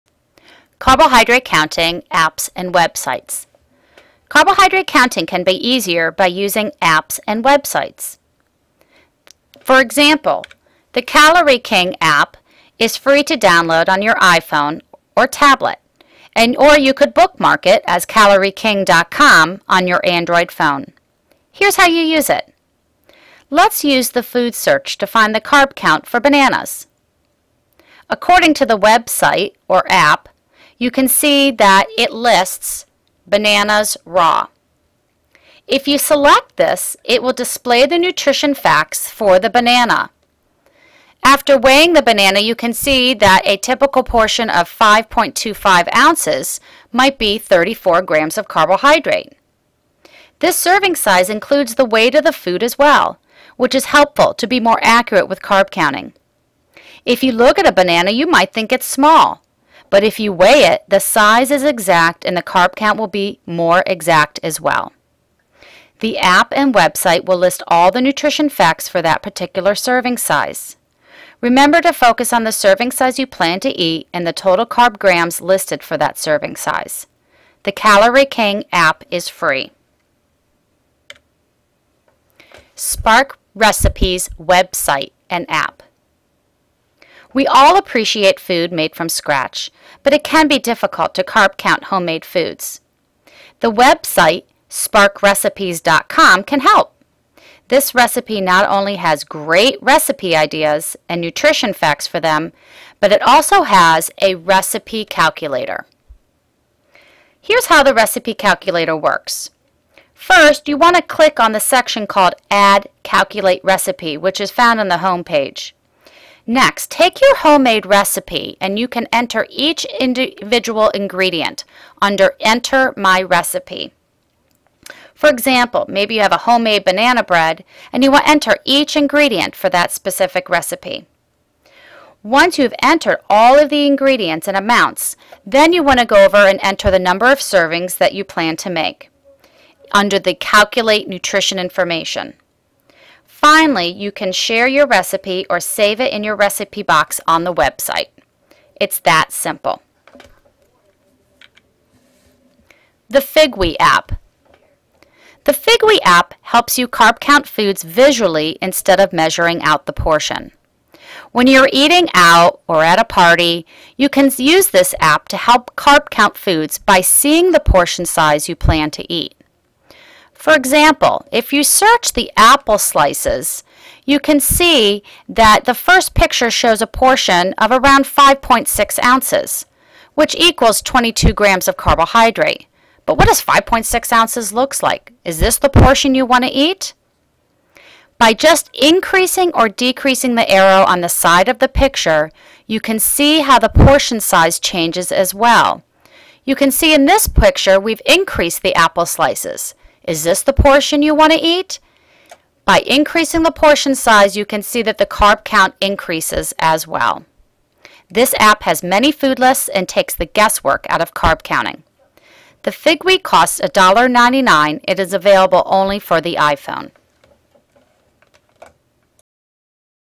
Using Carb Counting Apps and Websites
In this video, you will learn about the many apps and websites you can use to make counting carbohydrates easier.